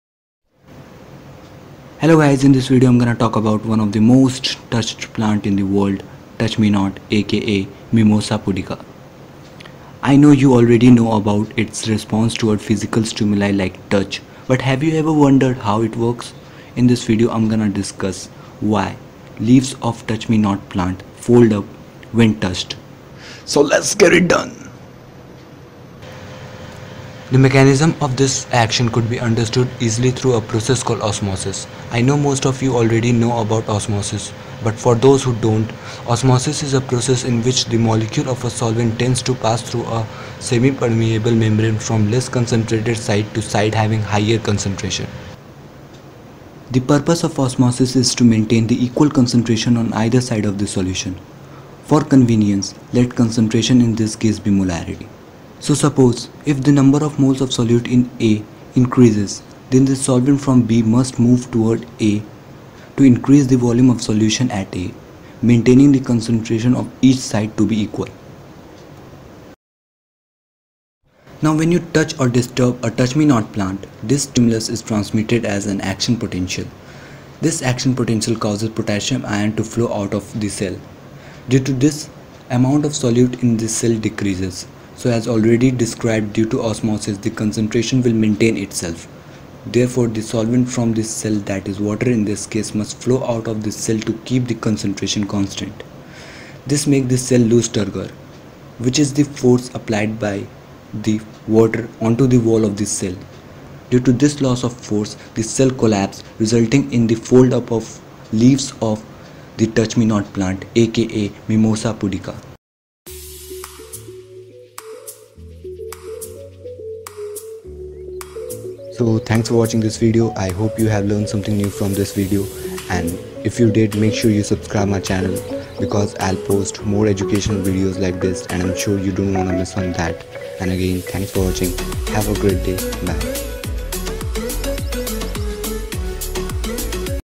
Shy plant | Sensitive plant | Touch me not | Mimosa pudica | Explained
Mimosa pudica -- from Latin: pudica "shy, bashful or shrinking"; also called sensitive plant, sleepy plant, Dormilones, touch-me-not, or shy plant-- is a creeping annual or perennial herb of the pea family Fabaceae often grown for its curiosity value: the compound leaves fold inward and droop when touched or shaken, defending themselves from harm, and re-open a few minutes later.The species is native to South America and Central America, but is now a pantropical weed. It can also be found in Asia in countries such as Bangladesh, Thailand, India, Indonesia, Malaysia, Philippines, and Japan. It grows mostly in undisturbed shady areas, under trees or shrubs.

Video structure

Introduction:- 0:00
Osmosis:- 0:31
Explanation of its response toward touch:- 1:10